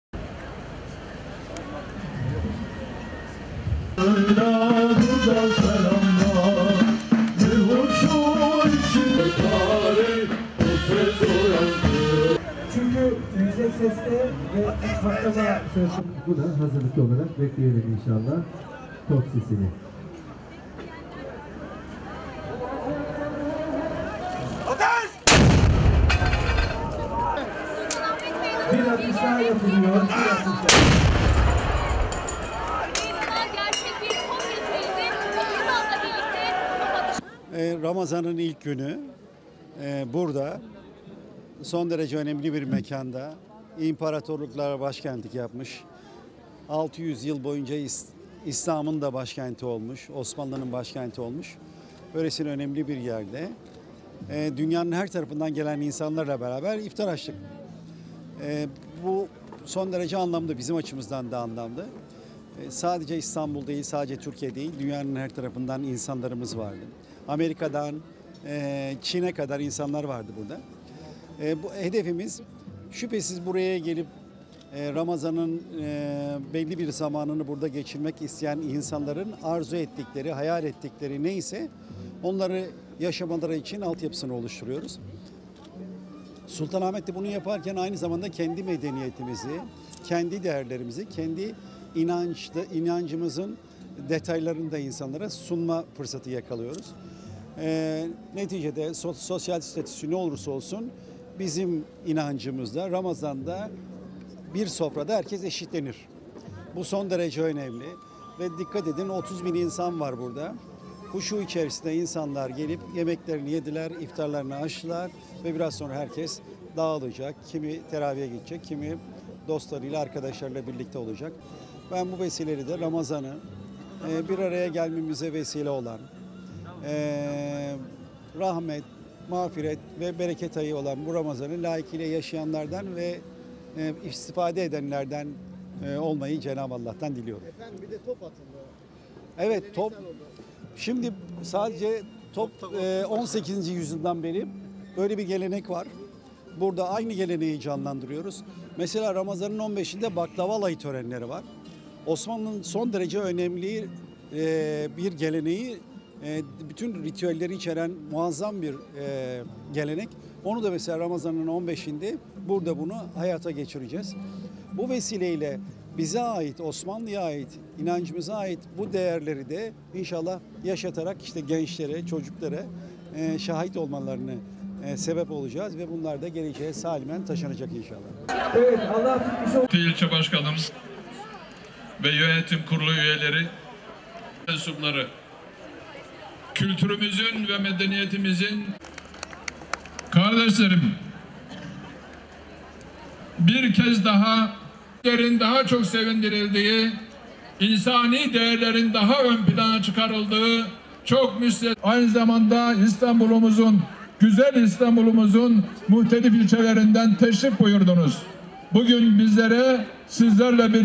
Sultanahmet'te 30 Bin Kişi Top Atışıyla İlk Oruçlarını Açtı
Fatih Belediyesi tarafından Sultanahmet Meydanı’nda düzenlenen programda bir araya gelen yaklaşık 30 bin kişi, Belediye Başkanı Mustafa Demir’in top atışını gerçekleştirmesiyle ilk oruçlarını açtı.

Fatih Belediyesi tarafından Sultanahmet Meydanı'nda düzenlenen programda bir araya gelen yaklaşık 30 bin kişi, Belediye Başkanı Mustafa Demir'in top atışını gerçekleştirmesiyle ilk oruçlarını açtı. Program her yıl olduğu gibi bu Ramazan'da da yoğun ilgi gördü. Evlerinden iftarlıklarını getiren vatandaşlar parklarda ve yeşillikler üzerinde sofra açarken, yaklaşık 30 bin kişi Fatih belediye Başkanı Mustafa Demir'in top atışını gerçekleştirmesiyle oruçlarını açtı.
Top atışını gerçekleştiren Fatih Belediye Başkanı Mustafa Demir, “Ramazanın ilk günü burada son derece önemli bir mekanda, imparatorluklara başkentlik yapmış, 600 yıl boyunca İslamiyet’in de başkenti olmuş, Osmanlı’nın başkenti olmuş, böylesine önemli bir yerde dünyanın her tarafından gelen insanlarla beraber iftar açtık. Bu bizim açımızdan son derece anlamlı. Sadece İstanbul değil, sadece Türkiye’den değil, dünyanın her tarafından insanlarımız var. Amerika’dan Çin’e kadar insanlar var burada. Hedefimiz şüphesiz buraya gelip ramazanın belli bir zamanlarını burada geçirmek isteyen insanların arzu ettikleri hayal ettikleri ne ise onları yaşamaları için onları yaşamaları için alt yapılarını oluşturuyoruz” ifadelerini kullandı.

“Bizim inancımızda bir sofrada herkes eşitlenir”

Sultanahmet Meydanı'nda gerçekleştirilen iftar programının önemine değinen Başkan Demir, “Sosyal statüsü ne olursa olsun bizim inancımızda bizim soframızda herkes eşitlenir. Bu son derece önemli ve dikkat edin 30 bin insan var burada. Huşu içerisinde insanlar gelip yemeklerini yediler. İftarlarını açtılar ve biraz sonra herkes dağılacak kimi teraviye gidecek kimi dostlarıyla arkadaşlarıyla beraber olacak” dedi.

Top atışının önemine de değinen Başkan Demir, “18. yüzyıldan beri böyle bir gelenek var. Burada aynı geleneği canlandırıyoruz. Ramazanın 15’inde baklava alayı törenleri var. Osmanlının son derece önemli bir geleneği, bütün ritüelleri içeren muazzam bir gelenek. Onu da ramazanın 15’inde burada hayata geçireceğiz. Bu vesileyle bize ait Osmanlıya ait geleneği de yaşatarak gençlere çocuklara şahit olmalarına sebep olacağız” şeklinde konuştu.